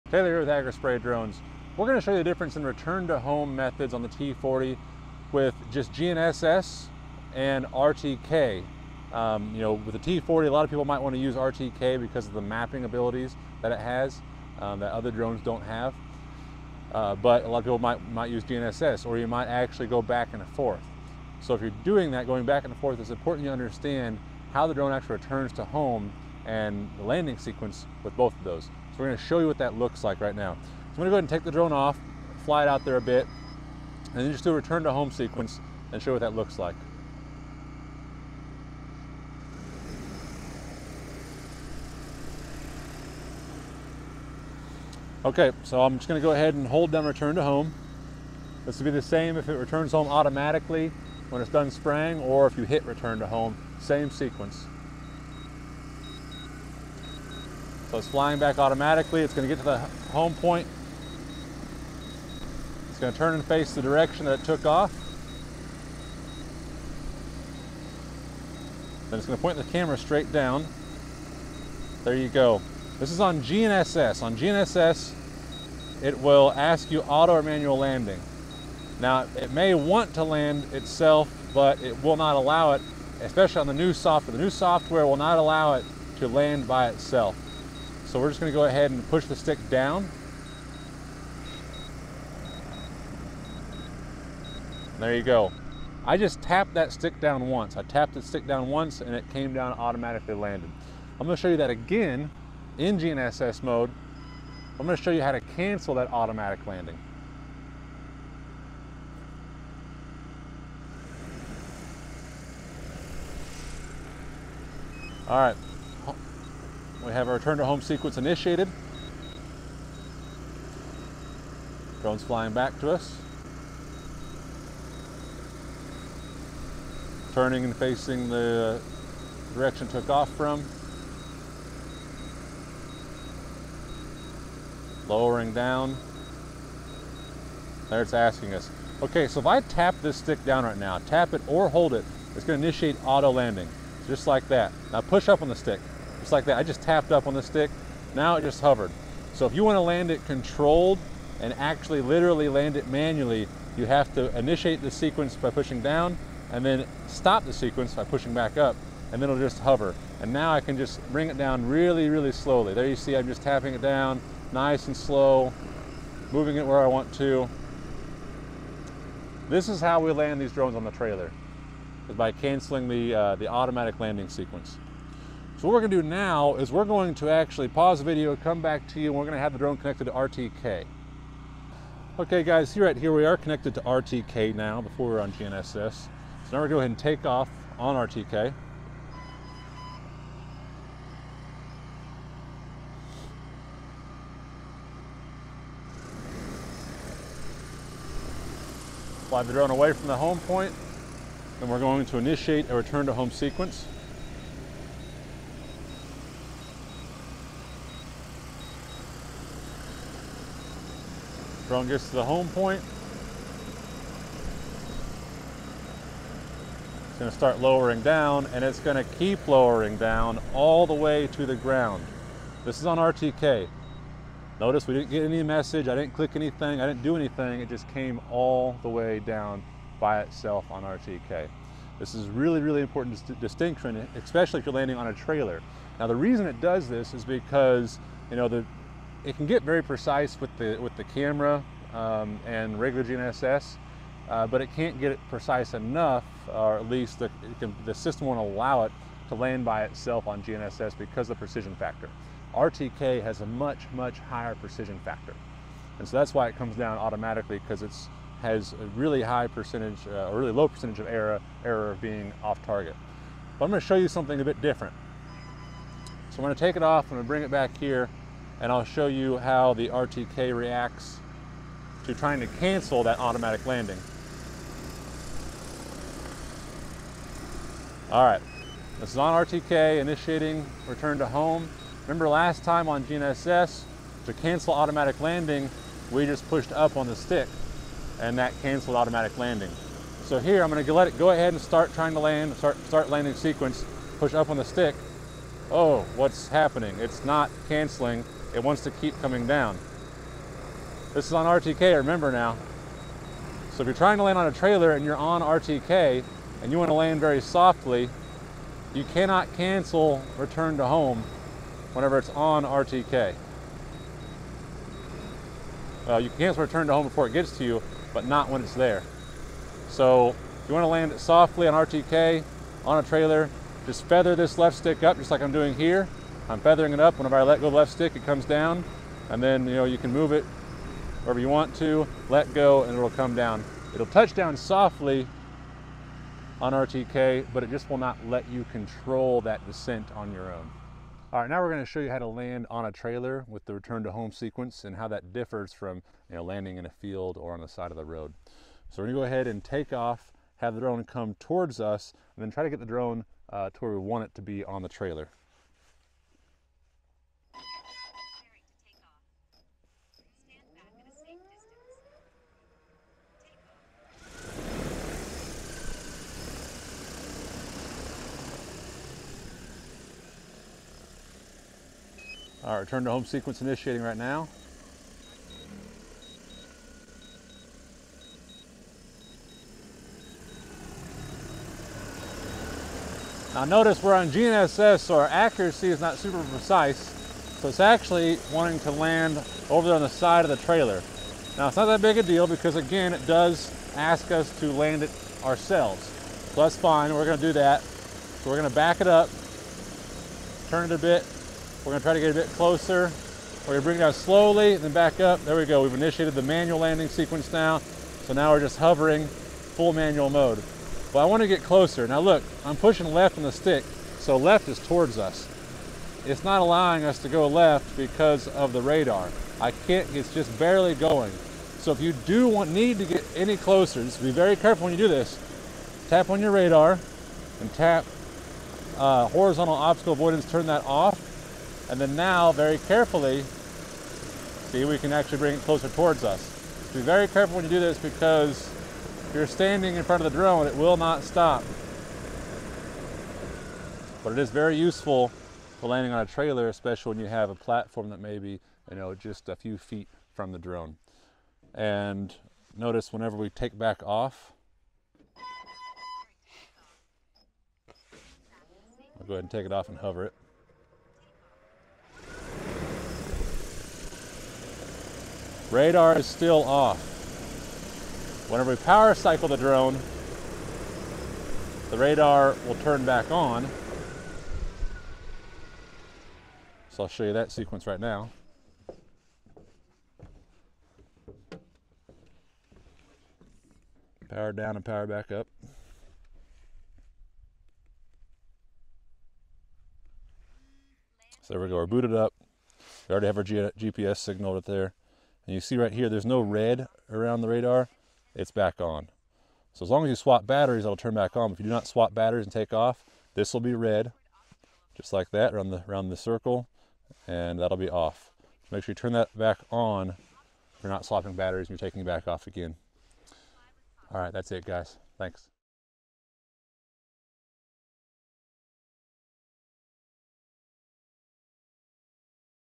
Return to Home | DJI Agras T40 | Agri Spray Drones
In this film, we will show the capabilities of the Agras T40's return to home feature. Join us as we explore how the drone seamlessly navigates its way back home and some of the methods the drone uses to do so.